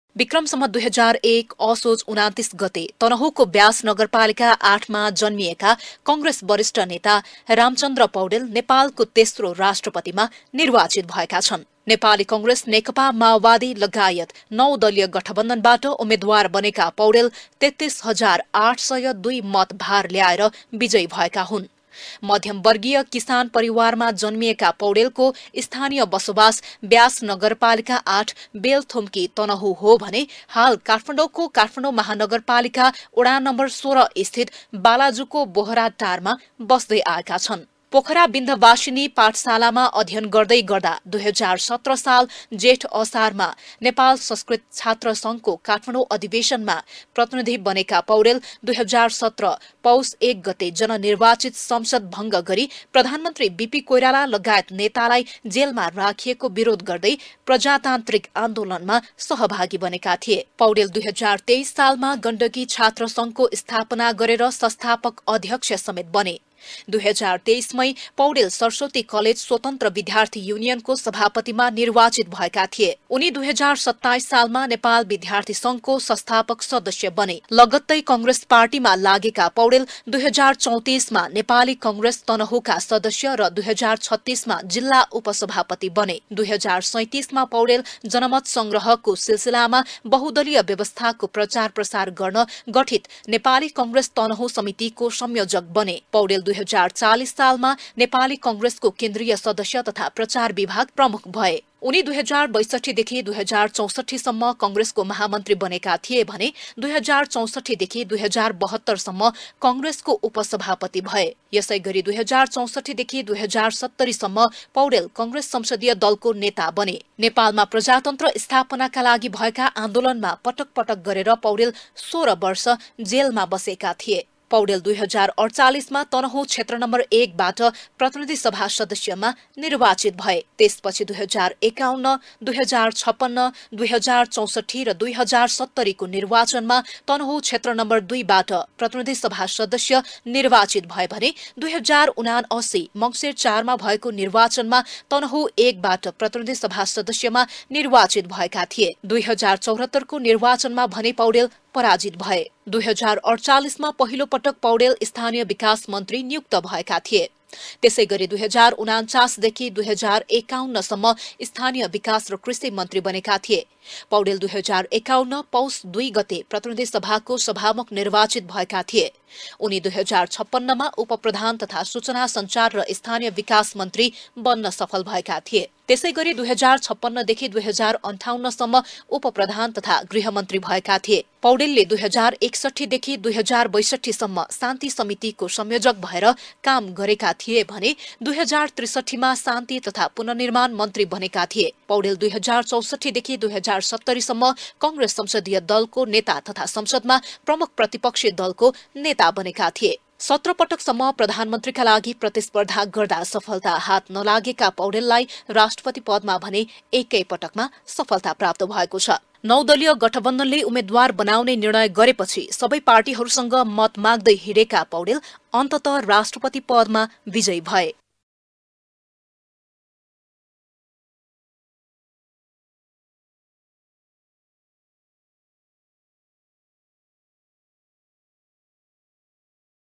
रामचन्द्र पौडेलको राजनीतिक यात्रा|बिद्यार्थी नेताहुँदै राष्ट्रपतिमा निर्वाचित हुँदासम्म गरेका संर्घष
रामचन्द्र पौडेलको राजनीतिक यात्रा | बिद्यार्थी नेता हुँदै राष्ट्रपतिमा निर्वाचित हुँदा सम्म गरेका संर्घषहरु